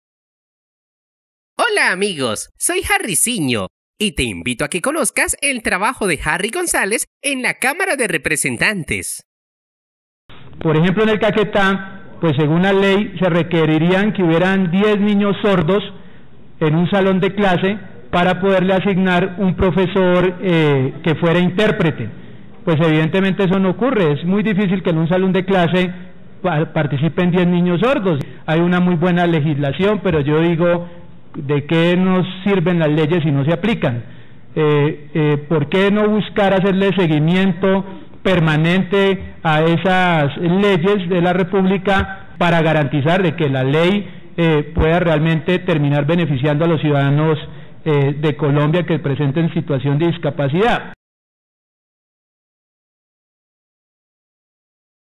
HARRY GONZALEZ defiende a la población con Capacidades Diversas.
El  Representante HARRY GONZÁLEZ hace un llamado al Gobierno Nacional para que exista una Política Publica de inclusión para la población con Capacidades Diversas